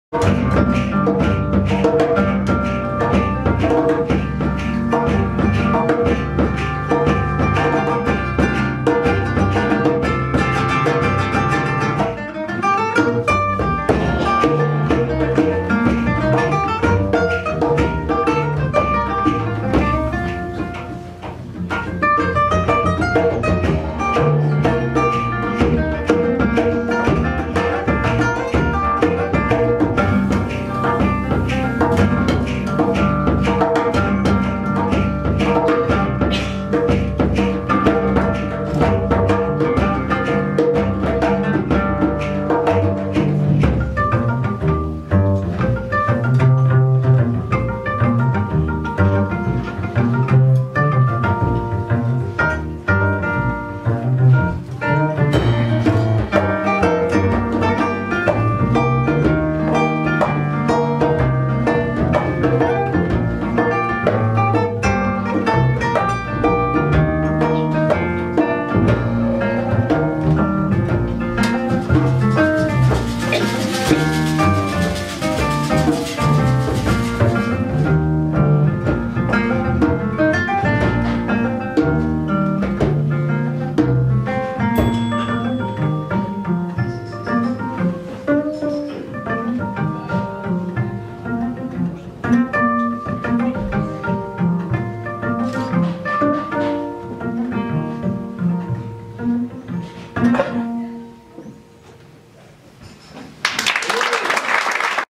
Leán Trio in 't Bruggenhuis
Fragment van het Leán trio in 't Bruggenhuis met
 Silke Clarysse - gitaar
Otto Kint - bas
Gielis Cautaers - percussie